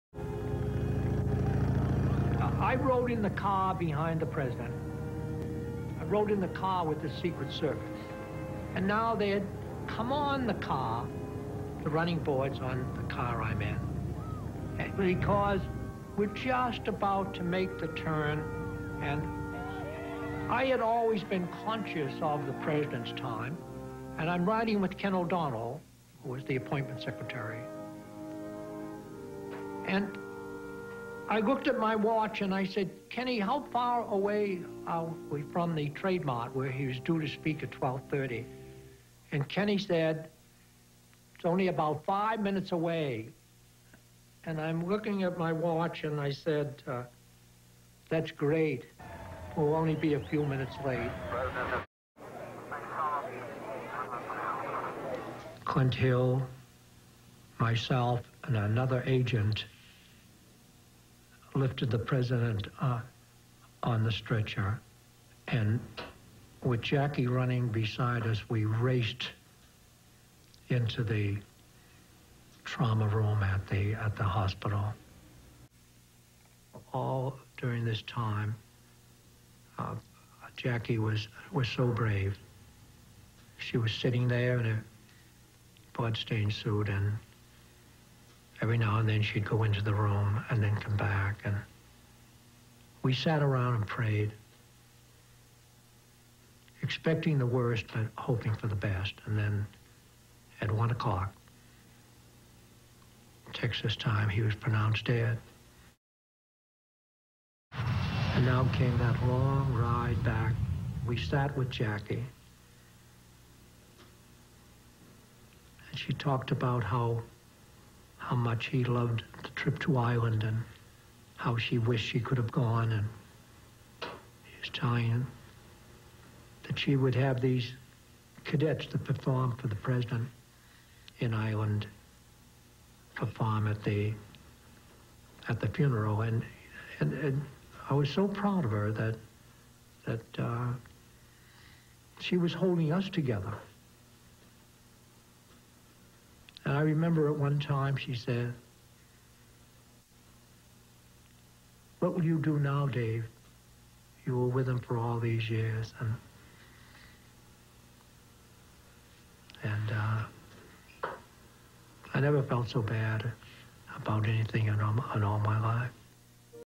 Dave Powers Interview 1
Dave Powers, special assistant to President John F. Kennedy.